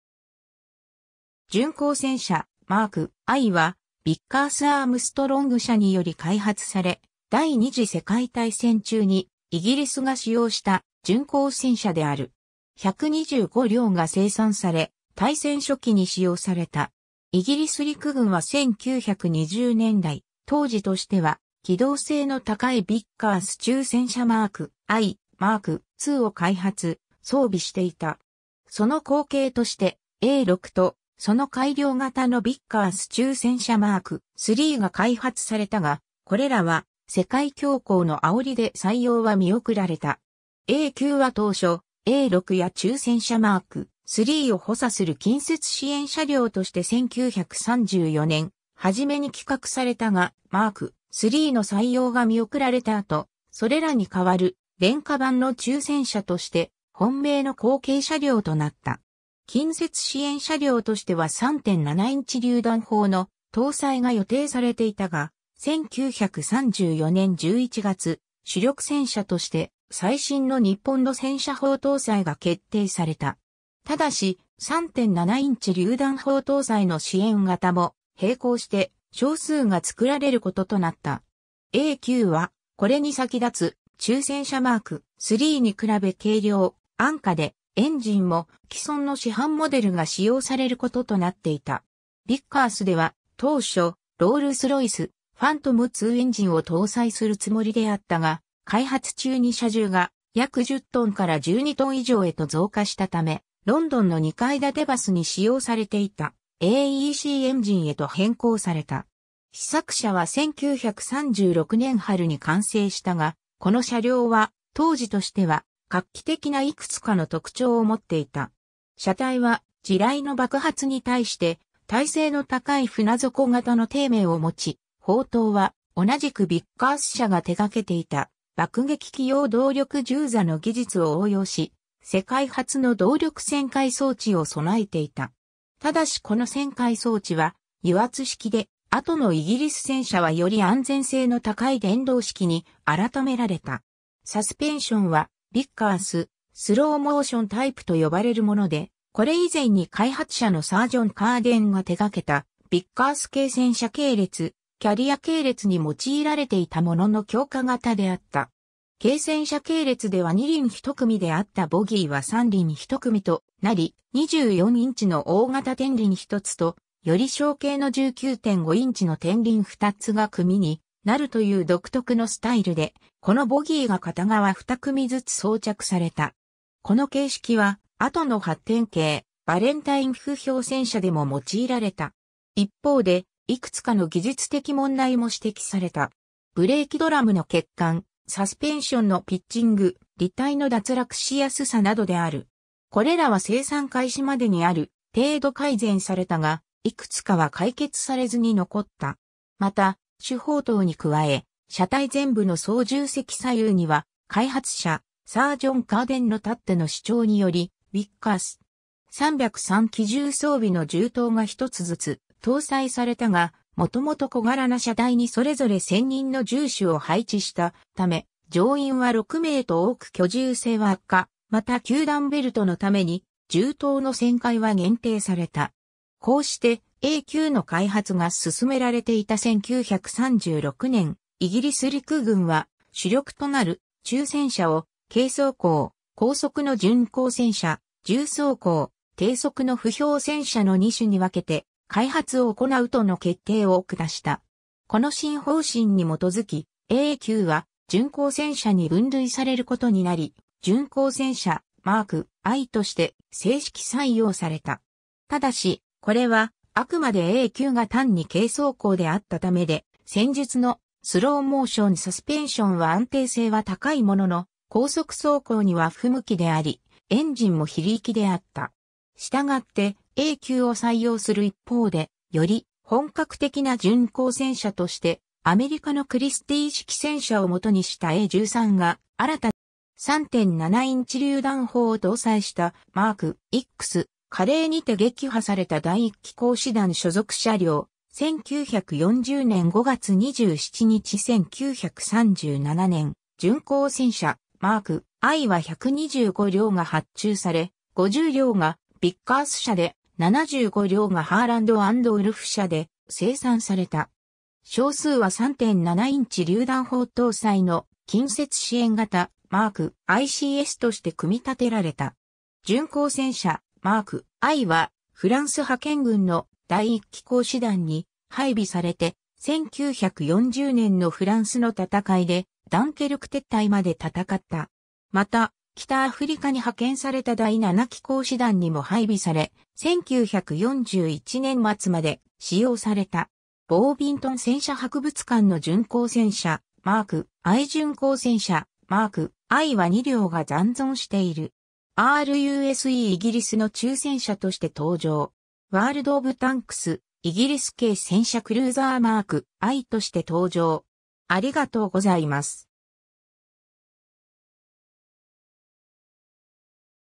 巡航戦車 Mk.I
巡航戦車 Mk.I（じゅんこうせんしゃ マーク1、Tank, Cruiser Mk.I（A9））は、ヴィッカース・アームストロング社により開発され、第二次世界大戦中にイギリスが使用した巡航戦車である。
125輌が生産され、大戦初期に使用された。
 イギリス陸軍は1920年代、当時としては機動性の高いヴィッカース中戦車 Mk.I、Mk.IIを開発・装備していた。
その後継として、A6（16トン戦車）とその改良型のヴィッカース中戦車 Mk.IIIが開発されたが、これらは世界恐慌のあおりで採用は見送られた。
 A9は当初、A6や中戦車Mk.IIIを補佐する近接支援車両として1934年初めに企画されたが、Mk.IIIの採用が見送られた後、それらに代わる廉価版の中戦車として、本命の後継車両となった。
近接支援車両としては3.7インチ榴弾砲の搭載が予定されていたが、1934年11月、主力戦車として最新の2ポンド（40mm）戦車砲搭載が決定された。
ただし、3.7インチ榴弾砲搭載の支援型も並行して少数が作られることとなった。
 A9は、これに先立つ中戦車Mk.IIIに比べ軽量・安価で、エンジンも既存の市販モデルが使用されることとなっていた。
ヴィッカースでは当初、ロールス・ロイス「ファントムII」エンジン（120馬力）を搭載するつもりであったが、開発中に車重が約10トンから12トン以上へと増加したため、ロンドンの2階建てバスに使用されていたAECエンジン（150馬力）へと変更された。
 試作車は1936年春に完成したが、この車両は、当時としては画期的ないくつかの特徴を持っていた。
車体は地雷の爆発に対して耐性の高い船底型の底面を持ち、砲塔は、同じくヴィッカース社が手がけていた爆撃機用動力銃座の技術を応用し、世界初の動力旋回装置を備えていた。
ただしこの旋回装置は油圧式で、後のイギリス戦車はより安全性の高い電動式に改められた。
 サスペンションはヴィッカース「スローモーション」タイプと呼ばれるもので、これ以前に開発者のサー・ジョン・カーデンが手掛けたヴィッカース軽戦車系列、キャリア系列に用いられていたものの強化型であった。
軽戦車系列では2輪1組であったボギーは3輪1組となり、24インチの大型転輪1つと、より小径の19.5インチの転輪2つが組になるという独特のスタイルで、このボギーが片側2組ずつ装着された。
この形式は、後の発展形バレンタイン歩兵戦車でも用いられた。
 一方で、いくつかの技術的問題も指摘された。
ブレーキドラムの欠陥、サスペンションのピッチング、履帯の脱落しやすさなどである。
これらは生産開始までにある程度改善されたが、いくつかは解決されずに残った。
また、主砲塔に加え、車体前部の操縦席左右には、開発者サー・ジョン・カーデンのたっての主張により、ヴィッカース.303機銃装備の銃塔が1つずつ搭載されたが、もともと小柄な車体にそれぞれ選任の銃手を配置したため、乗員は6名と多く居住性は悪化、また給弾ベルトのために銃塔の旋回は限定された。
 こうしてA9の開発が進められていた1936年、イギリス陸軍は、主力となる中戦車を軽装甲・高速の巡航戦車、重装甲・低速の歩兵戦車の2種に分けて開発を行うとの決定を下した。
この新方針に基づき、A9は巡航戦車に分類されることになり、巡航戦車 Mk.Iとして制式採用された。
ただし、これはあくまでA9が単に軽装甲であったためで、先述の「スローモーション」サスペンションは安定性は高いものの高速走行には不向きであり、エンジンも非力であった。
したがって、A9を採用する一方で、より「本格的」な巡航戦車として、アメリカのクリスティー式戦車をもとにしたA13（後の巡航戦車 Mk.III）が新たに開発されることとなった。
 3.7インチ榴弾砲を搭載したMk.ICS、カレー (フランス) にて撃破された第1機甲師団所属車両、1940年5月27日 1937年、巡航戦車 Mk.I は125輌が発注され、50輌がヴィッカース社で、75輌がハーランド・アンド・ウルフ社で生産された。
少数は3.7インチ榴弾砲搭載の近接支援（Close Support）型、Mk.I CSとして組み立てられた。
 巡航戦車 Mk.Iは、フランス派遣軍の第1機甲師団に配備されて、1940年のフランスの戦いでダンケルク撤退まで戦った。
また、北アフリカに派遣された第7機甲師団にも配備され、1941年末まで使用された。
 ボービントン戦車博...